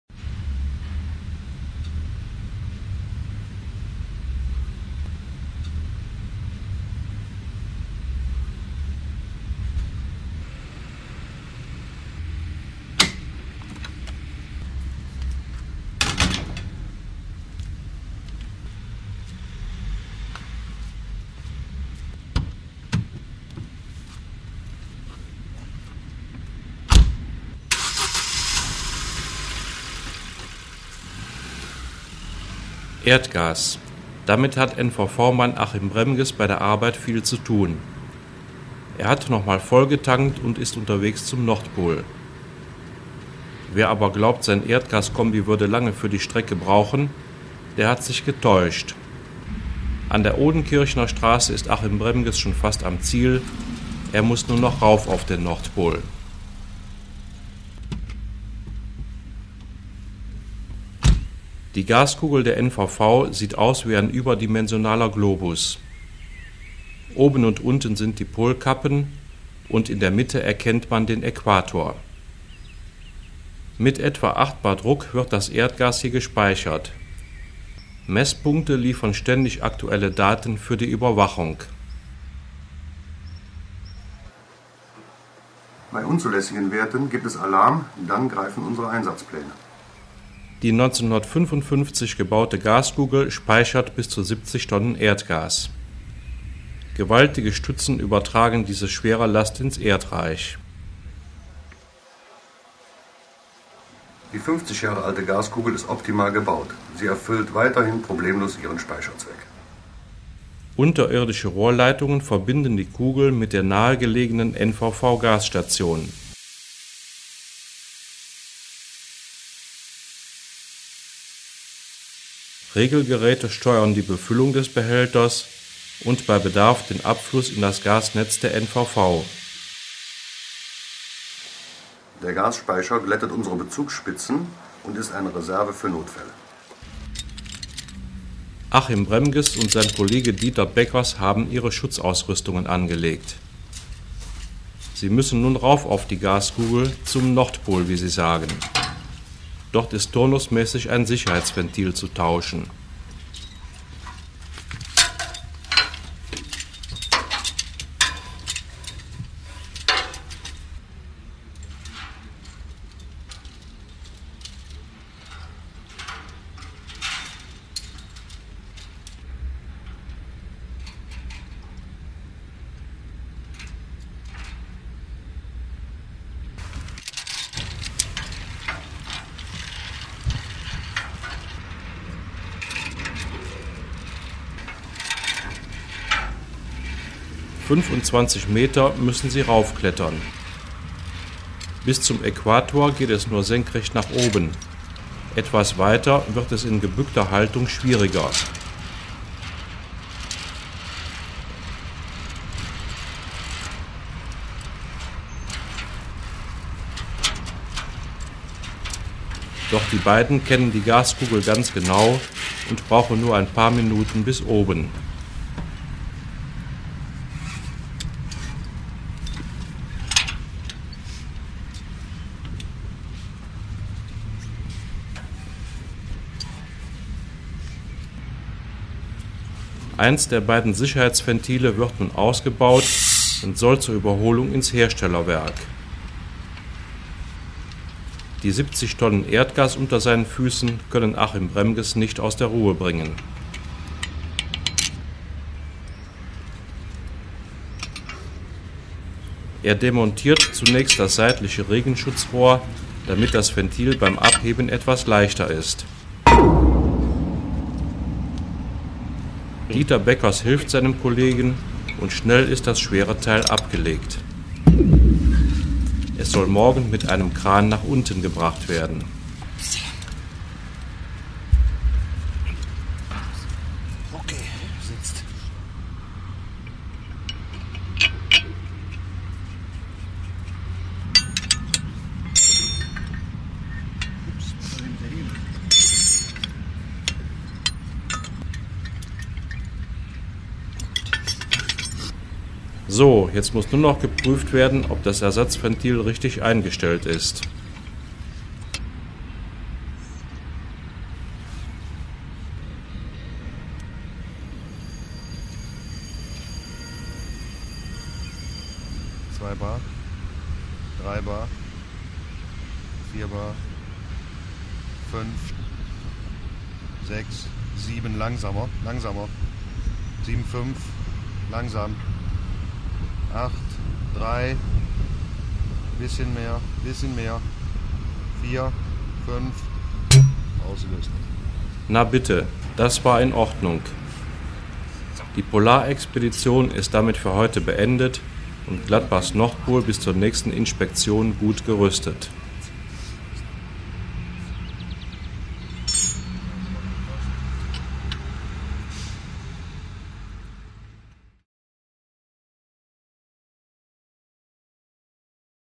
Unter Druck: Gladbachs Nordpol
Mönchengaldbach: Austausch eines Druckventils am Gas-Behälter
Der Film von Hans-Georg Grombein erreichte in Wettbewerben:
2006: 2. Preis beim Landes-Filmfestival
2006: Bronze beim Bundesfilmfestival Reportagefilm